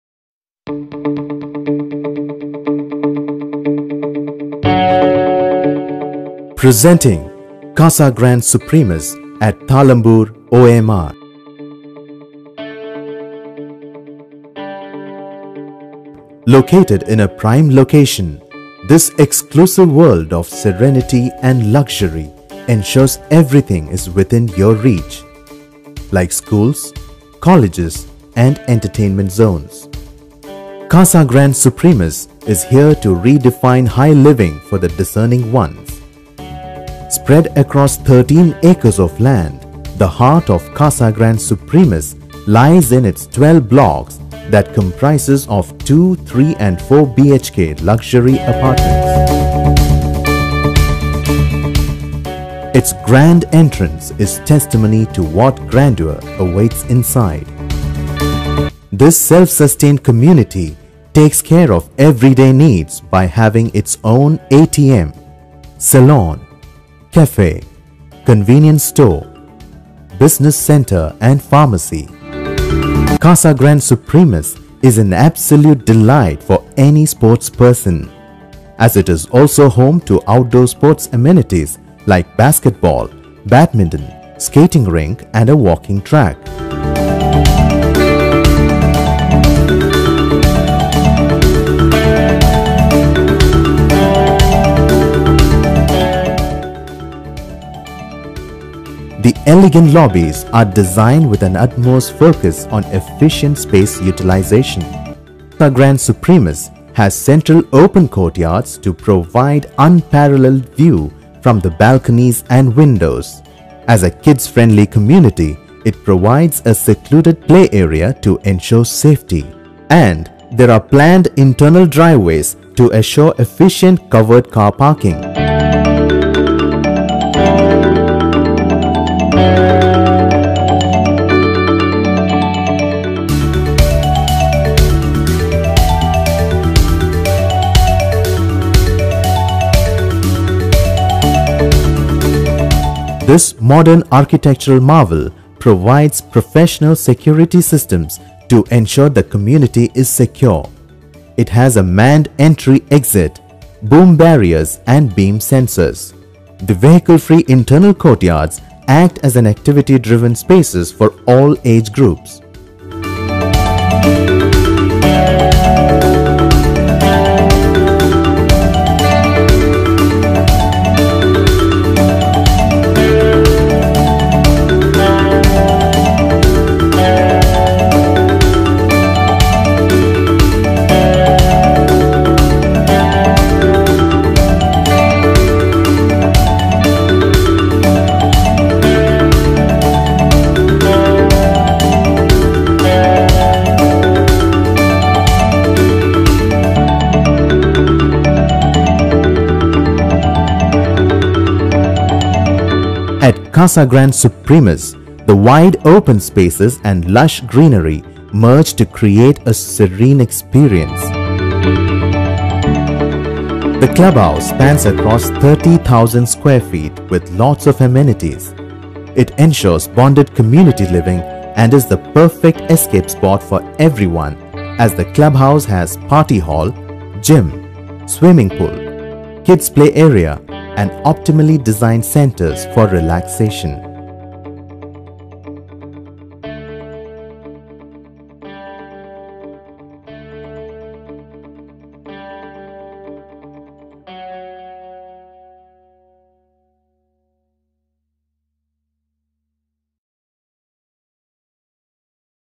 Casagrand Supremus Walkthrough/OMR/Navalur
728 lifestyle apartments (456 in Phase I)
spread across 12.65 acresStilt + 4 design
structure Contemporary new age architecture
Apartment loaded with lifestyle features at never heard of price Efficiently planned internal spaces for optimum space utilization
deal for investment and yielding good rentals Abundant ventilation Vasthu compliant Amenities and features like swimming pool, clubhouse, gym & many more Signature apartments exclusively in Blocks 6 & 14